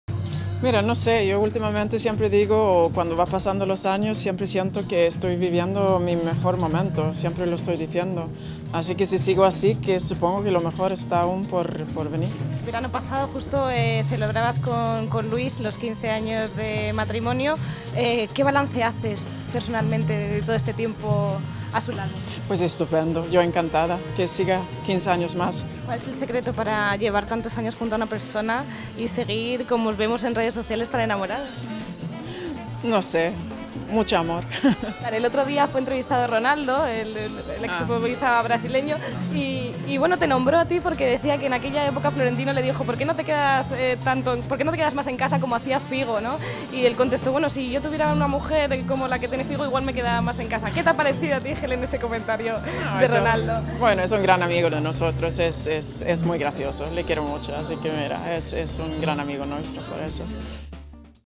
Helen Sweden cuenta el secreto de su amor con Luis Figo | Diez Minutos
La mujer de Luis Figo confiesa que el amor es el secreto para que su relación con el exfutbolista funcione.